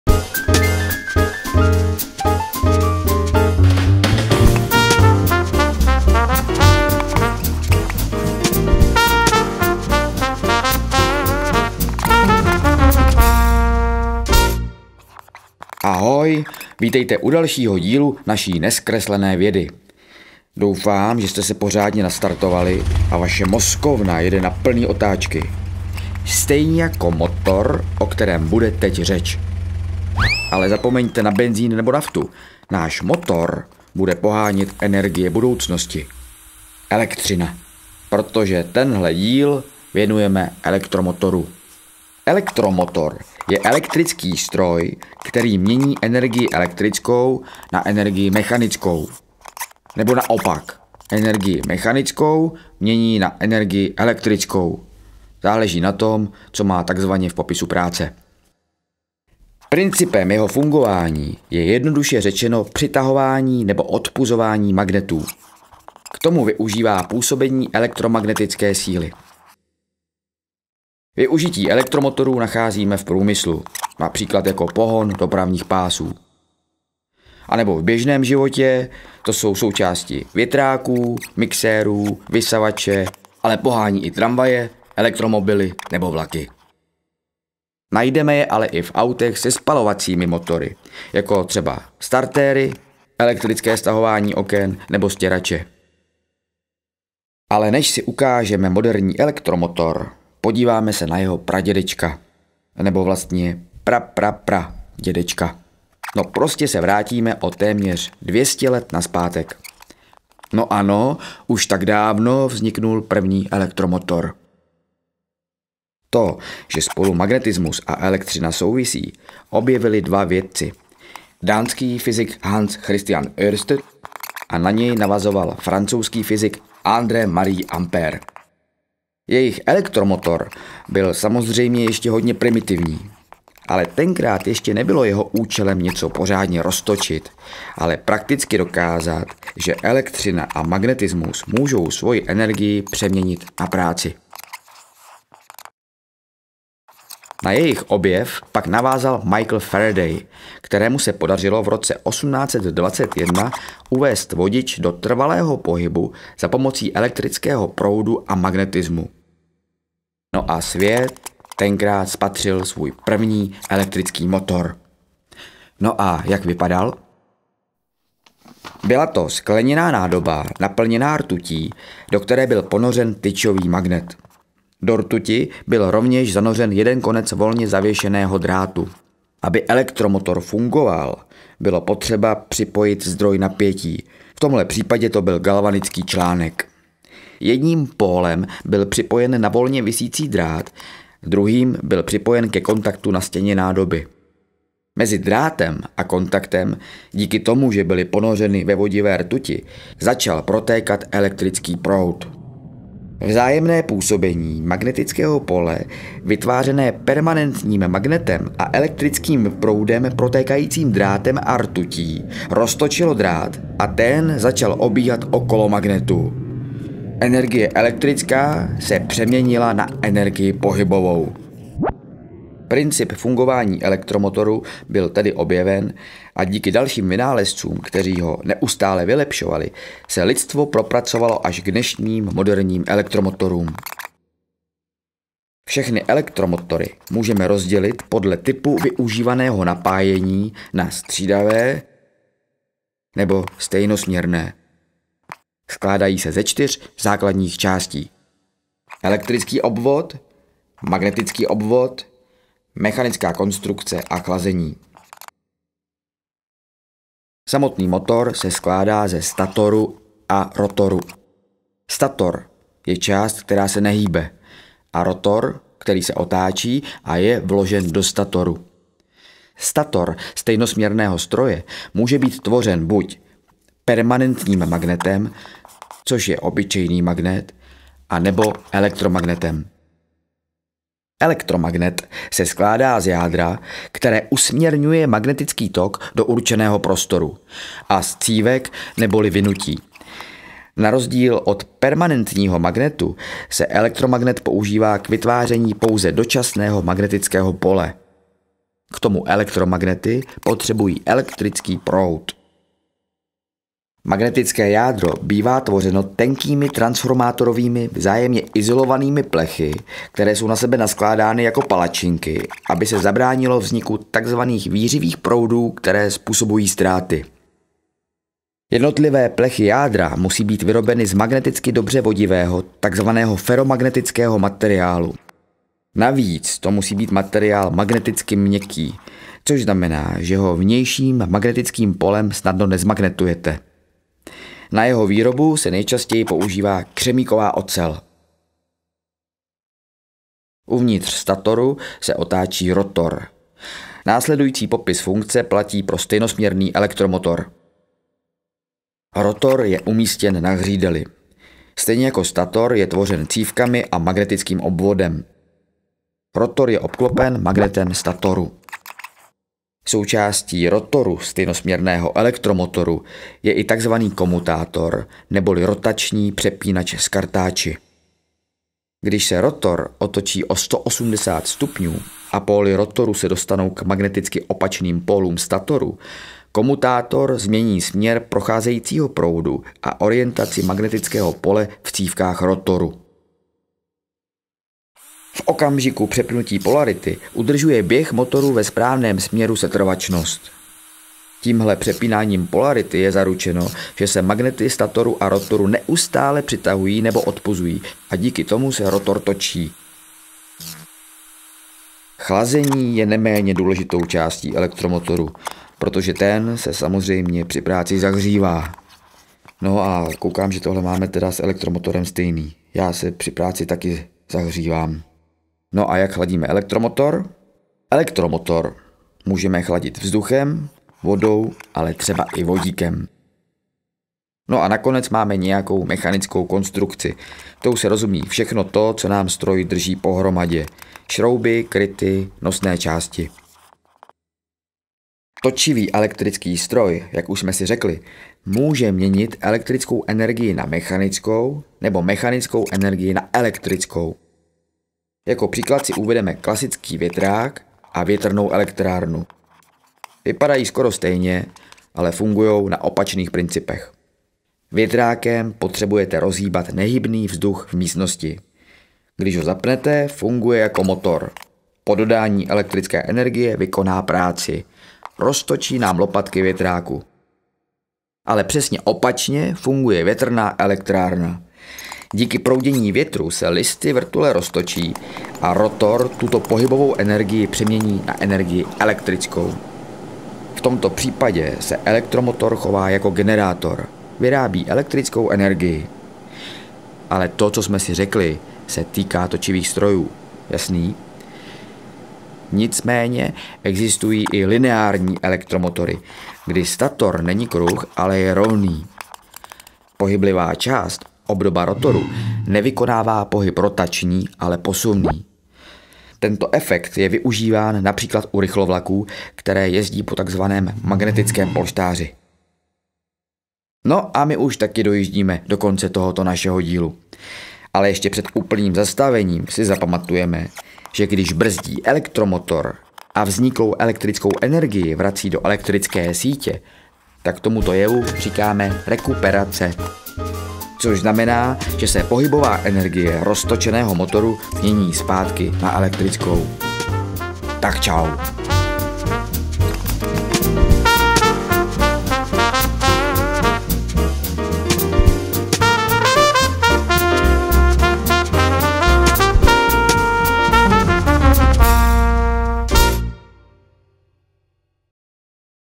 Jak funguje elektromotor – NEZkreslená věda III
NEZkreslená věda III: třetí série vzdělávacího cyklu Akademie věd České republiky.

Aktuální témata i vědecké evergreeny,
jak jste je dosud neviděli – totiž
NEZkresleně!

Odborným garantem dílu je doc. Ing. Miroslav Chomát, CSc.

Projekt vznikl za podpory Komerční banky, a. s.